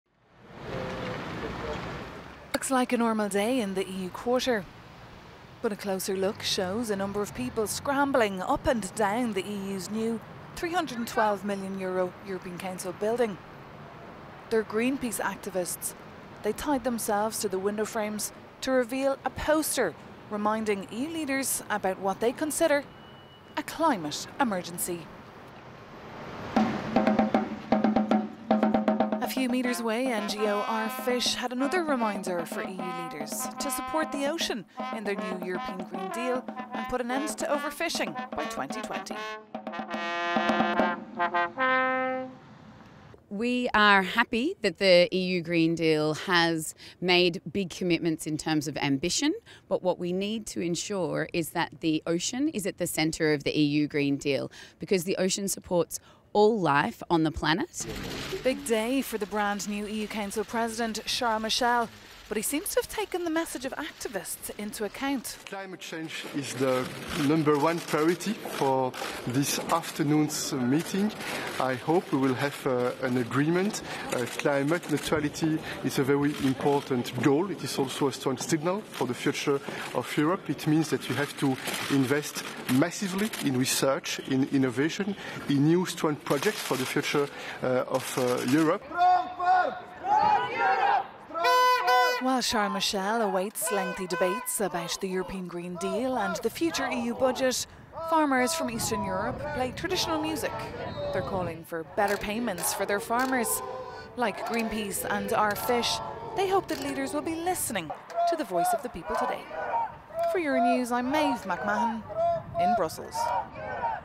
EU summit climate showdown
Europe's new Green Deal faces its first test as EU leaders meet in Brussels with the aim of agreeing to carbon neutrality by 2050, but not every country is on board ahead of talks.…